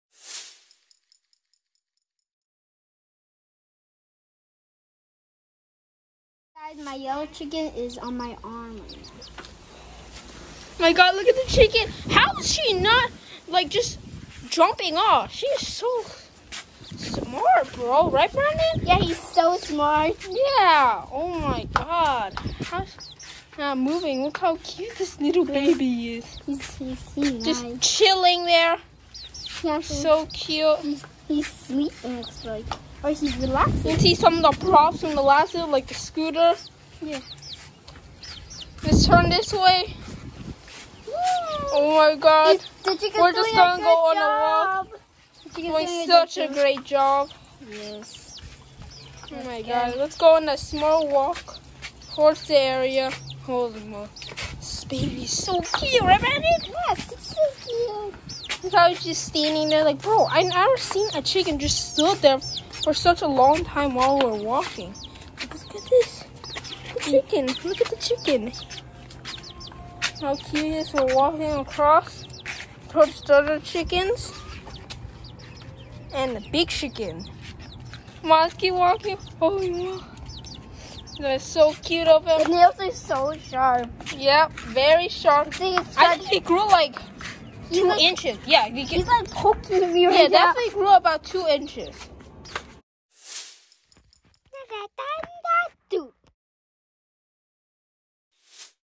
Going on a walk with my yellow chicken that is on my arm!! (Cool)!
Today I am going on a peaceful walk when our yellow chick and jumped on my arm. And it did not jump down just wanted to play me.!!!🌳🪨🐣🐤😎😎😆🤣